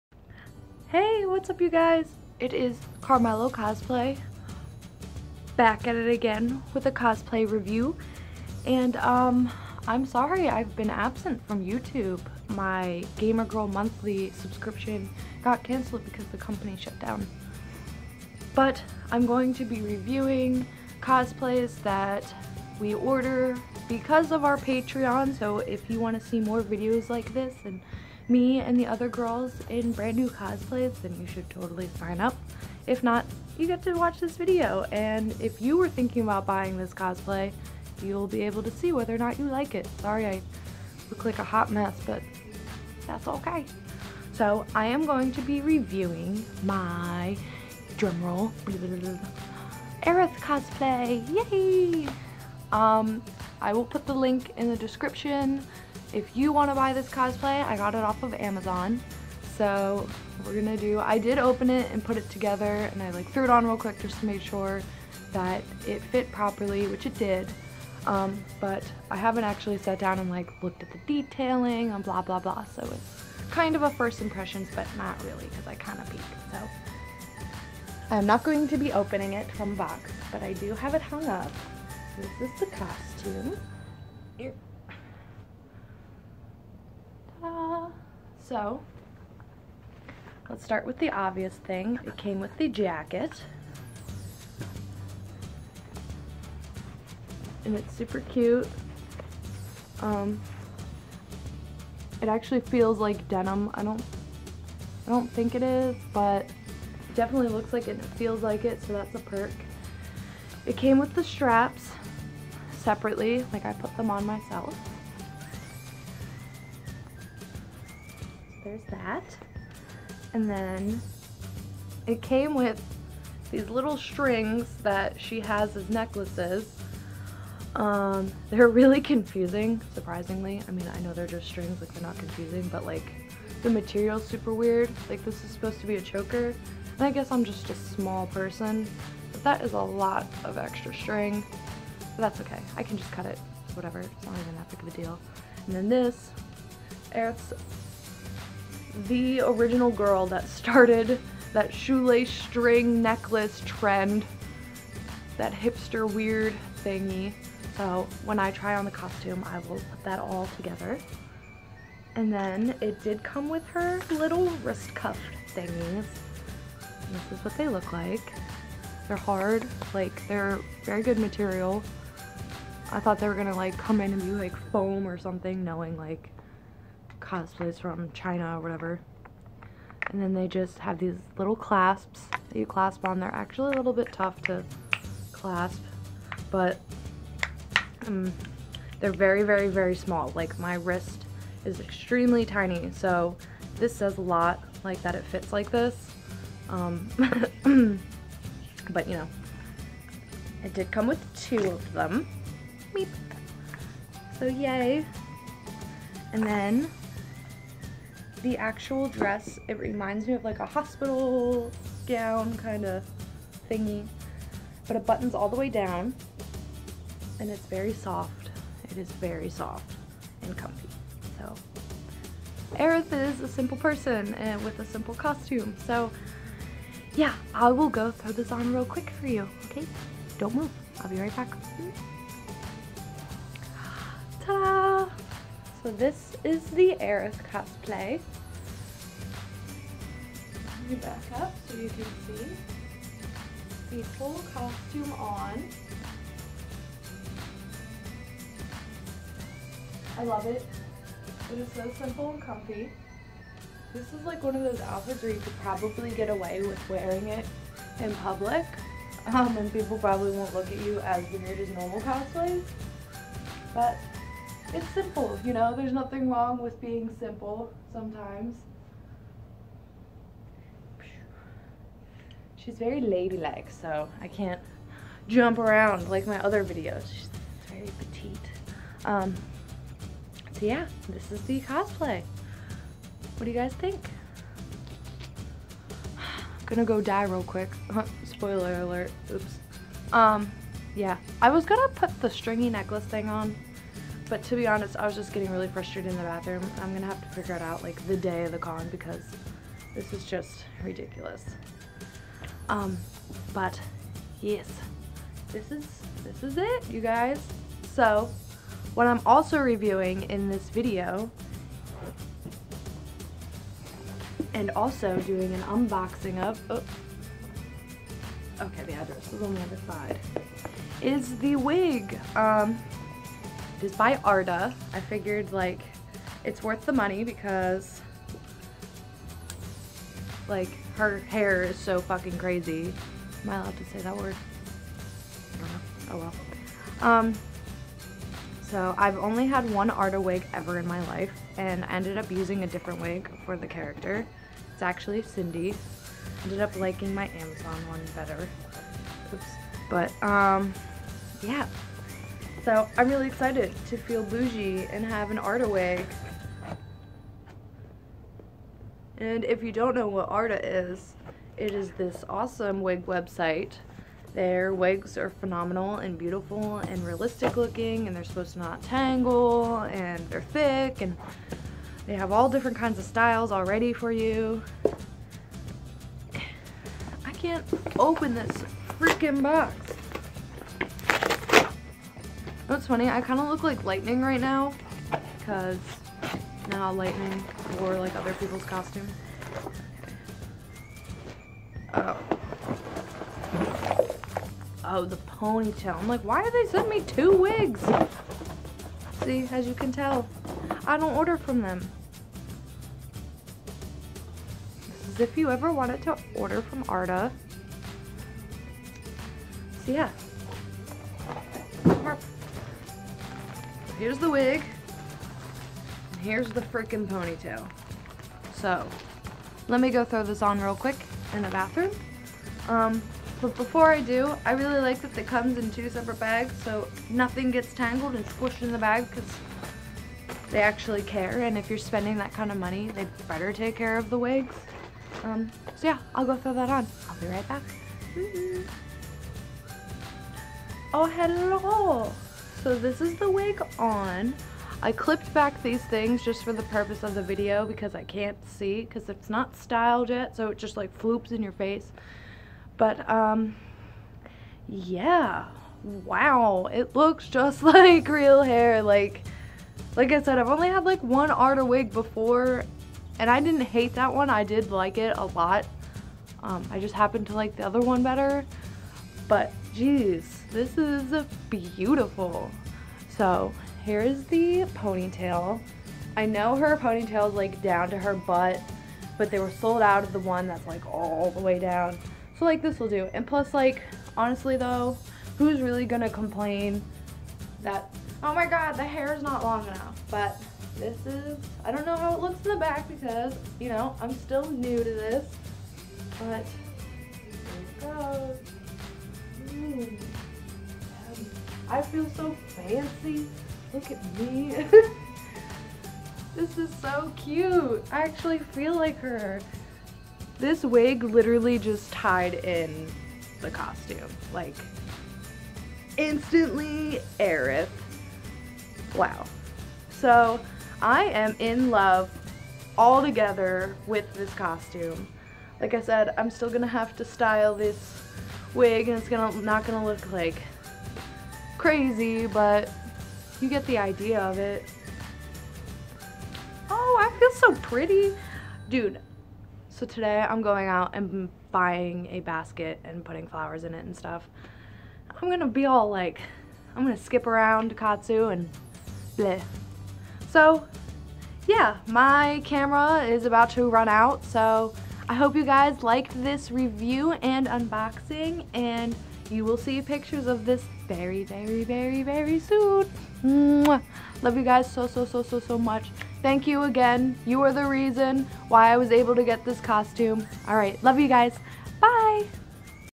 Cosplay Review with Caram3llo Cosplay - Aerith
We're changing things up in the new year. We're moving away from mystery boxes (mainly because we have a ton of crap on our shelves now, only half of it, we actually care about), to cosplay reviews.

Let's face it. We love building our own stuff, but sometimes we just don't have time. So we are going to be reviewing cosplays. Sometimes they'll be from cosplay companies, sometimes from Amazon, but we'll give you an idea of whether you should pick it up or save your money.

Remember, cosplay is for everyone. If you think you're less of a cosplayer because it's bought instead of made, don't bother commenting. We'll either ban you or ignore you.

DAZCOS US Size Adult Women's VII Aerith Gainsborough 1st Cosplay Costume

Wig by Arda:
Jeanie Classic - Small 21"